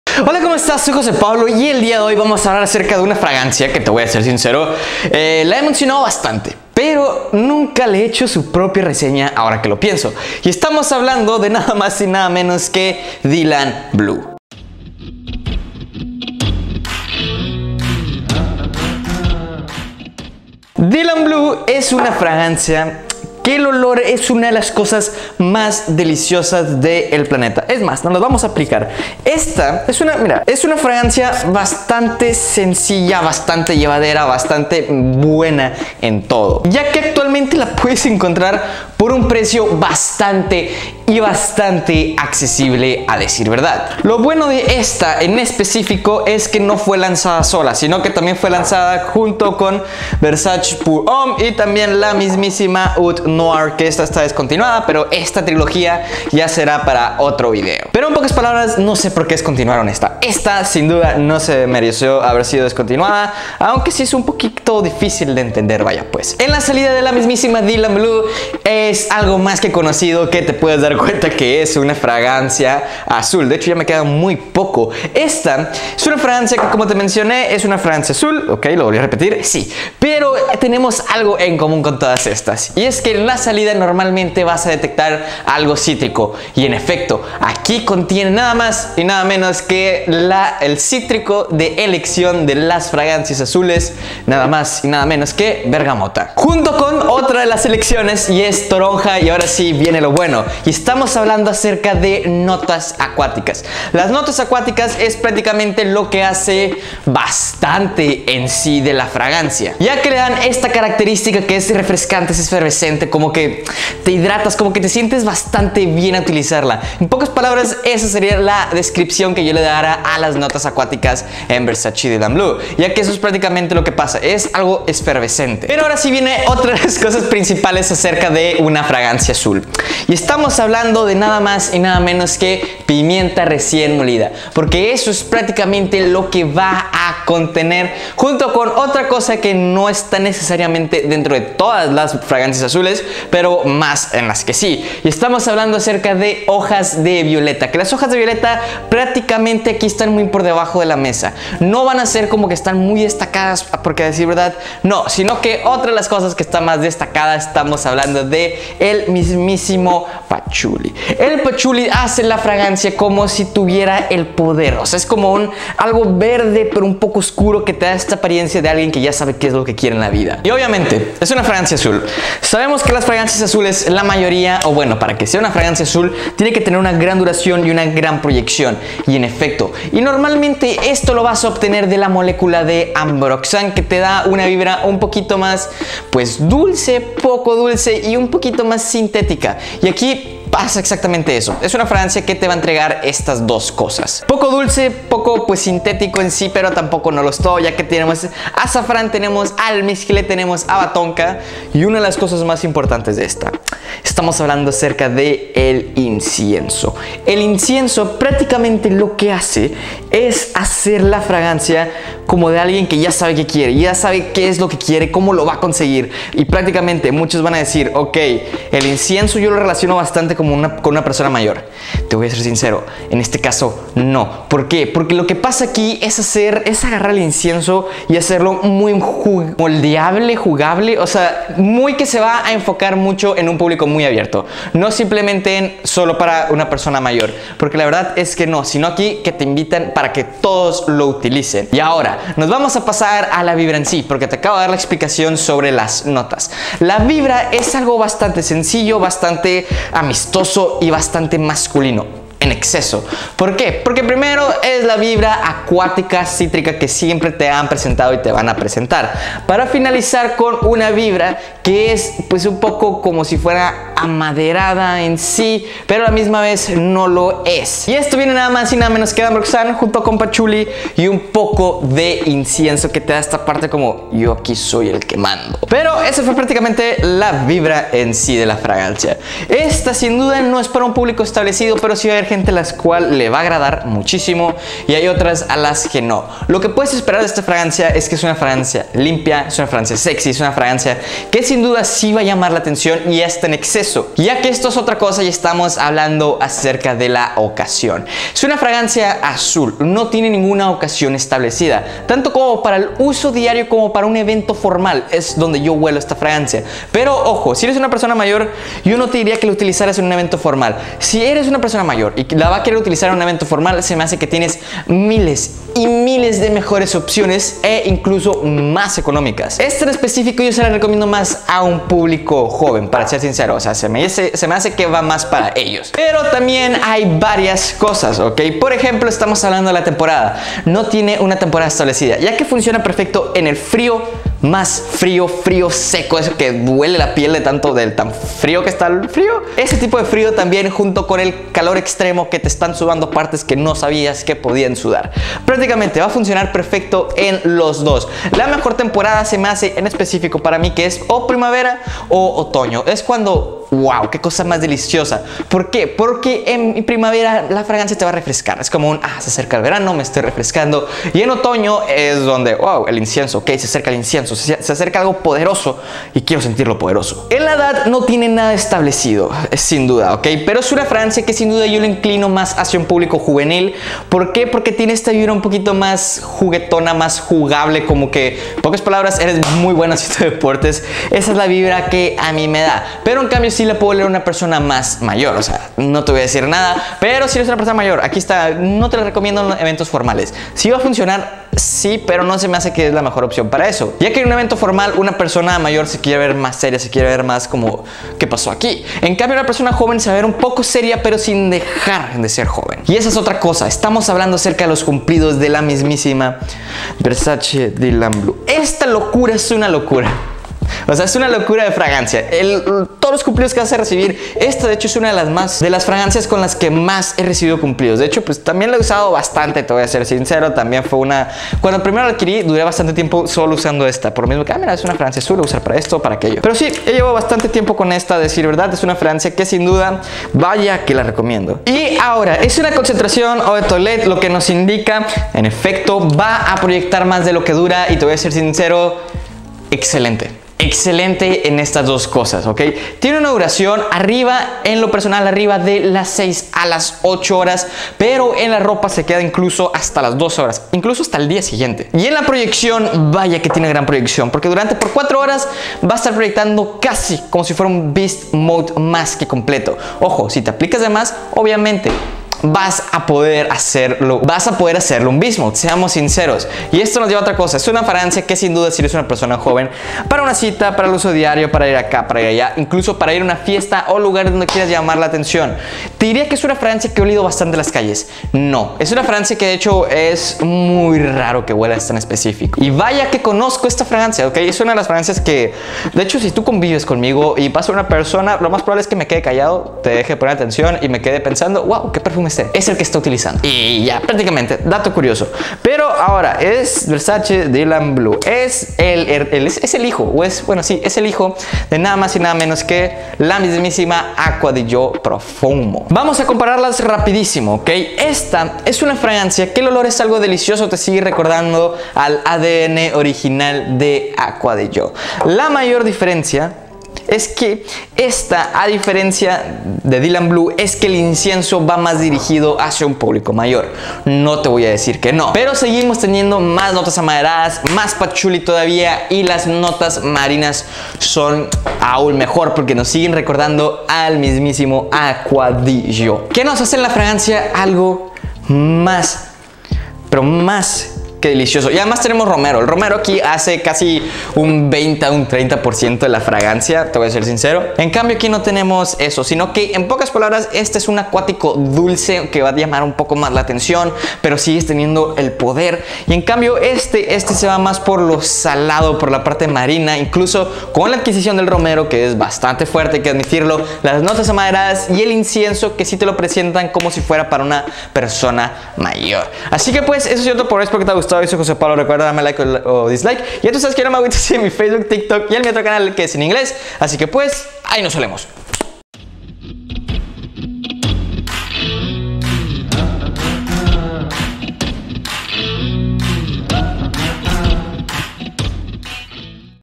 VERSACE - DYLAN BLUE "LA FRAGANCIA POR ELECCIÓN!" // PP
Hola!!! Soy Jose Pablo y hoy te hablo sobre VERSACE DYLAN BLUE!! una de la mejores fragancias azules!! AHÍ NOS OLEMOS!!!